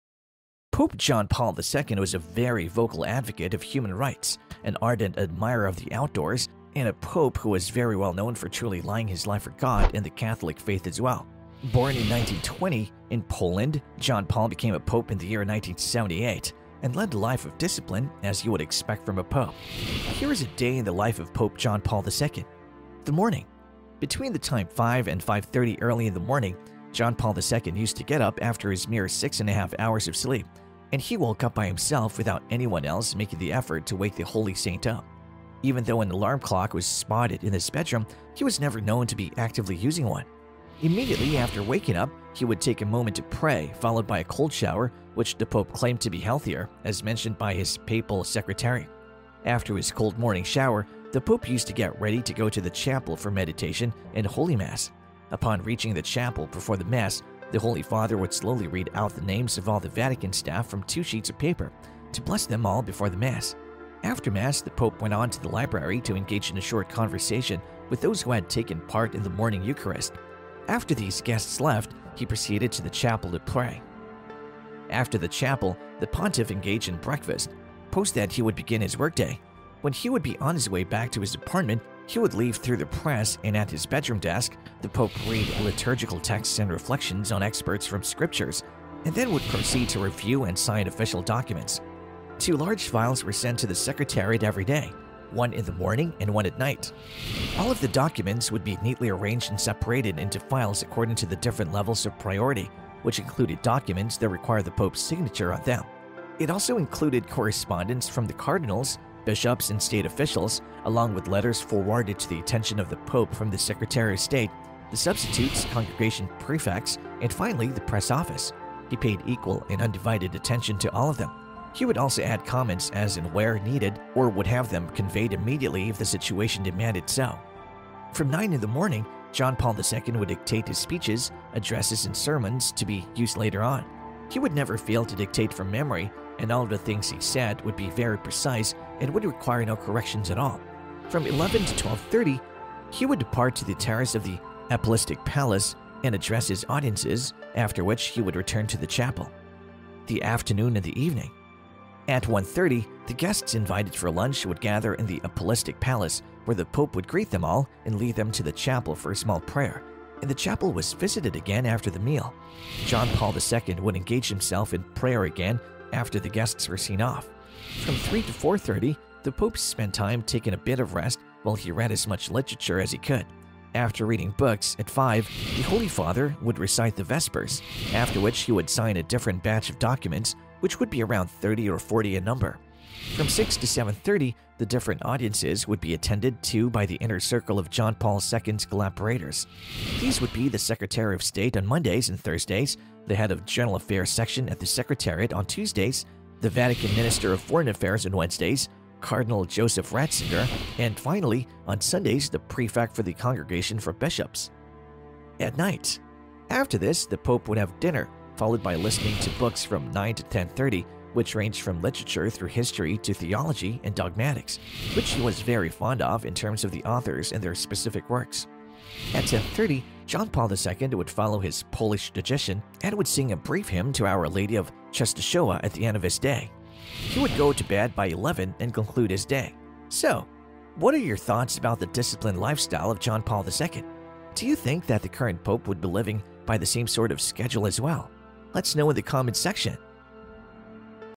A day in the life of a Pope John Paul II
Pope John Paul II was a very vocal advocate of human rights, an ardent admirer of the outdoors, and a pope who was very well known for truly lying his life for god and the catholic faith as well.

Born in 1980, in Poland, John Paul became a pope in the year 1978, and led a life of discipline, as you would expect from a pope. Here is a day in the life of Pope John Paul II:
The Morning
Between the time five and five-thirty early in the morning, John Paul II used to get up after his mere six and a half hours of sleep, and he woke up by himself, without anyone else making the effort to wake the holy saint up. Even though an alarm clock was spotted in his bedroom, he was never known to be actively using one.

Immediately after waking up, he would take a moment to pray, followed by a cold shower, which the Pope claimed to be healthier, as mentioned by his papal secretary. After his cold morning shower, the Pope used to get ready to go to the chapel for meditation and Holy Mass.

Upon reaching the chapel before the Mass, the Holy Father would slowly read out the names of all the Vatican staff from two sheets of paper, to bless them all before the Mass. After Mass, the Pope went on to the library to engage in a short conversation with those who had taken part in the morning Eucharist. After these guests left, he proceeded to the chapel to pray.

After the chapel, the Pontiff engaged in breakfast, post that he would begin his workday. When he would be on his way back to his apartment, he would leaf through the press, and at his bedroom desk, the Pope read liturgical texts and reflections on excerpts from Scripture, and then would proceed to review and sign official documents. Two large files were sent to the Secretariat every day, one in the morning and one at night.

All of the documents would be neatly arranged and separated into files according to the different levels of priority, which included documents that required the Pope’s signature on them. It also included correspondence from the cardinals, bishops, and state officials, along with letters forwarded to the attention of the Pope from the Secretary of State, the substitutes, congregation prefects, and finally, the press office. He paid equal and undivided attention to all of them, he would also add comments as and where needed or would have them conveyed immediately if the situation demanded it so.

From 9 in the morning, John Paul II would dictate his speeches, addresses, and sermons to be used later on. He would never fail to dictate from memory, and all of the things he said would be very precise and would require no corrections at all. From 11 to 12:30, he would depart to the terrace of the Apolistic Palace and address his audiences, after which he would return to the Chapel.
The Afternoon and the Evening:
At 1:30, the guests invited for lunch would gather in the Apolistic palace, where the Pope would greet them all and lead them to the chapel for a small prayer, and the chapel was visited again after the meal. John Paul II would engage himself in prayer again after the guests were seen off.

From 3:00 to 4:30, the Pope spent time taking a bit of rest while he read as much literature as he could. After reading books, at 5, the Holy Father would recite the Vespers, after which he would sign a different batch of documents, which would be around thirty or forty in number.

From 6:00 to 7:30, the different audiences would be attended to by the inner circle of John Paul II’s collaborators. These would be the Secretary of State on Mondays and Thursdays, the Head of General Affairs Section at the Secretariat on Tuesdays, the Vatican minister of foreign affairs on Wednesdays, Cardinal Joseph Ratzinger, and finally on Sundays the Prefect for the Congregation for Bishops.
At Night:
After this, the Pope would have dinner, followed by listening to books from 9:00 to 10:30, which ranged from literature through history to theology and dogmatics, which he was very fond of in terms of the authors and their specific works.

At 10:30, John Paul II would follow his Polish tradition and would sing a brief hymn to Our Lady of Czestochowa at the end of his day. He would go to bed by 11 and conclude his day.

So, what are your thoughts about the disciplined lifestyle of John Paul II? Do you think that the current Pope would be living by the same sort of schedule as well?